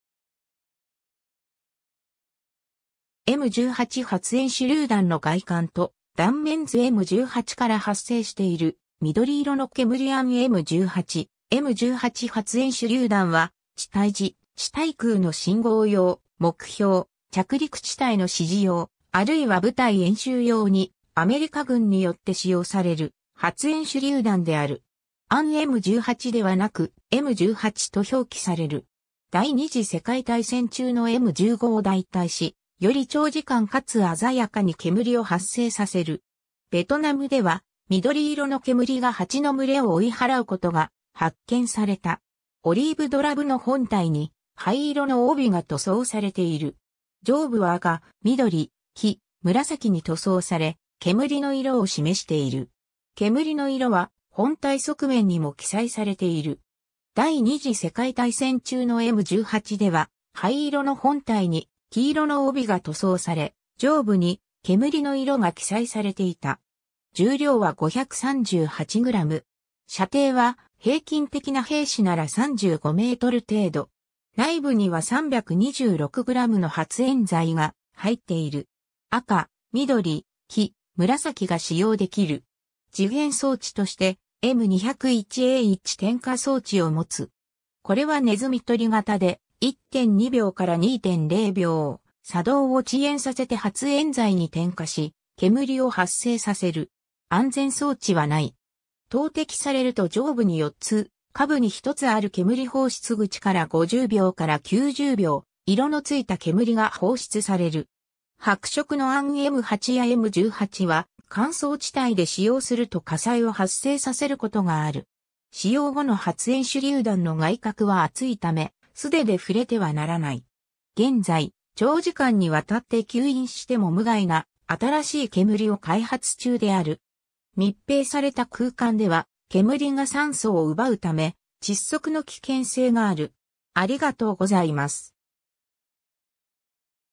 AN M18
M18発煙手榴弾の外観と断面図 M18から発生している緑色の煙 AN M18、M18発煙手榴弾 (M18 Smoke Grenade) は、地対地・地対空の信号用、目標・着陸地帯の指示用、あるいは部隊演習用にアメリカ軍によって使用される発煙手榴弾である。
「AN M18」ではなく「M18」と表記される。
第二次世界大戦中のM15を代替し、より長時間かつ鮮やかに煙を発生させる。
 ベトナムでは、緑色の煙がハチの群れを追い払うことが発見された。
 オリーブドラブの本体に灰色の帯が塗装されている。
上部は赤・緑・黄・紫に塗装され、煙の色を示している。
煙の色は本体側面にも記載されている。
第二次世界大戦中のM18では、灰色の本体に黄色の帯が塗装され、上部に煙の色が記載されていた。
 重量は538g 。
射程は平均的な兵士なら35m程度。
内部には326gの発煙剤が入っている 。
赤・緑・黄・紫が使用できる。
 時限装置として、M201A1点火装置を持つ。
これはねずみ取り型で、1.2秒から2.0秒、作動を遅延させて発煙剤に点火し、煙を発生させる。
安全装置は無い。
投擲されると上部に4つ、下部に1つある煙放出口から50秒から90秒、色の付いた煙が放出される。
 白色のAN-M8やM18は、乾燥地帯で使用すると火災を発生させることがある。
使用後の発煙手榴弾の外殻は熱いため、素手で触れてはならない。
現在、長時間にわたって吸引しても無害な新しい煙を開発中である。
密閉された空間では、煙が酸素を奪うため、窒息の危険性がある。